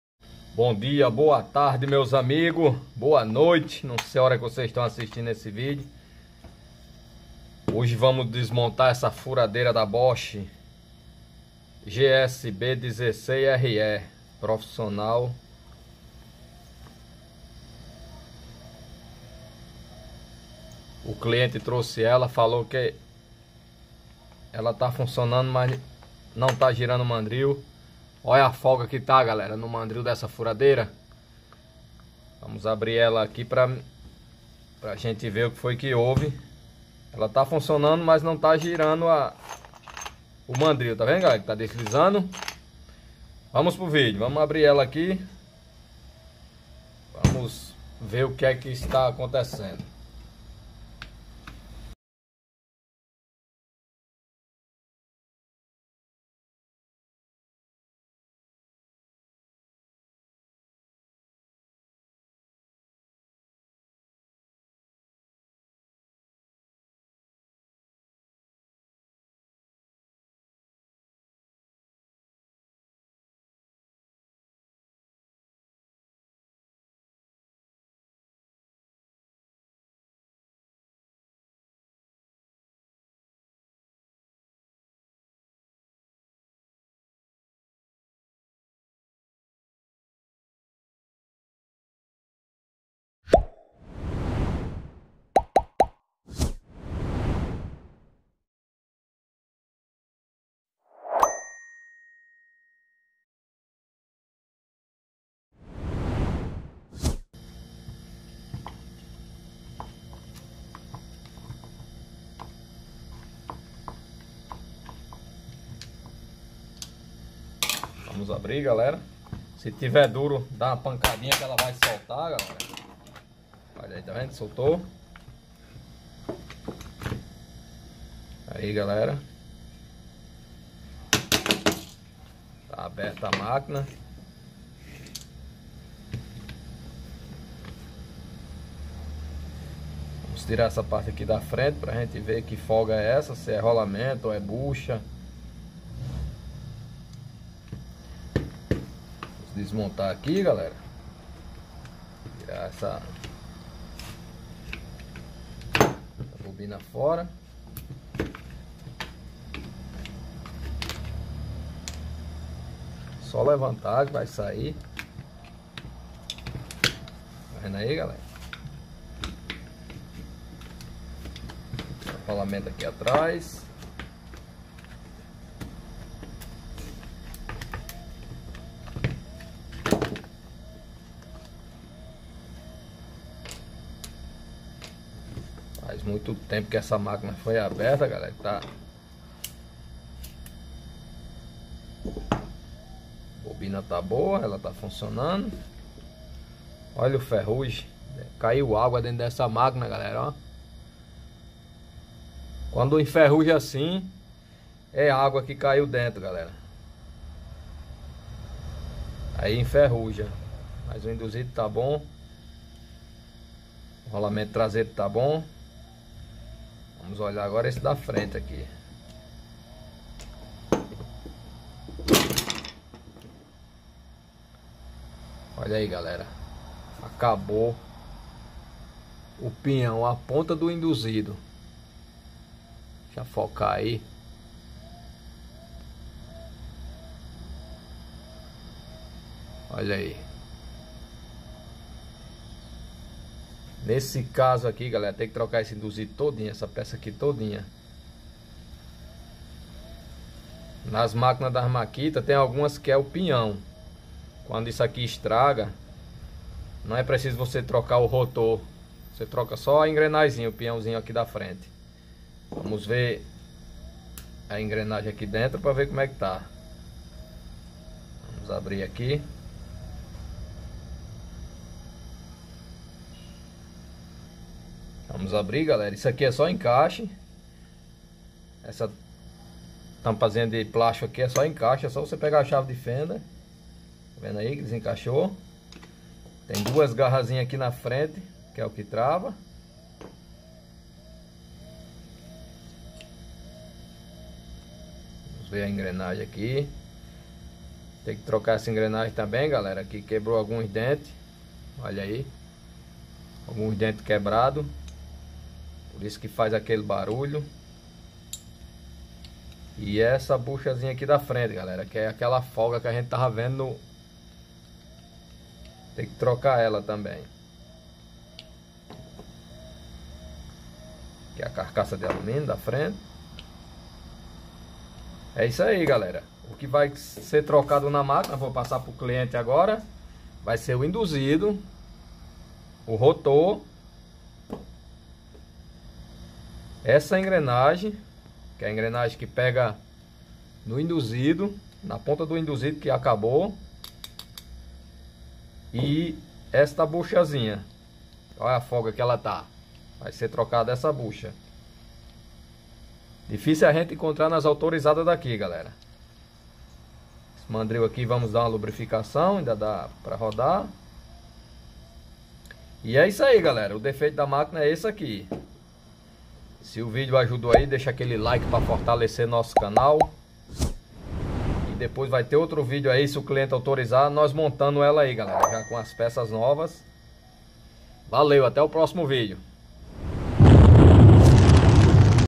COMO DESMONTAR FURADEIRA BOSCH GSB-16 RE
NESSE VIDEO MOSTRO COMO DESMONTAR A FURADEIRA BOSCH GSB-16 RE

GOSTARIA DE AJUDAR NOSSO CANAL COM QUALQUER VALOR CHAVE PIX (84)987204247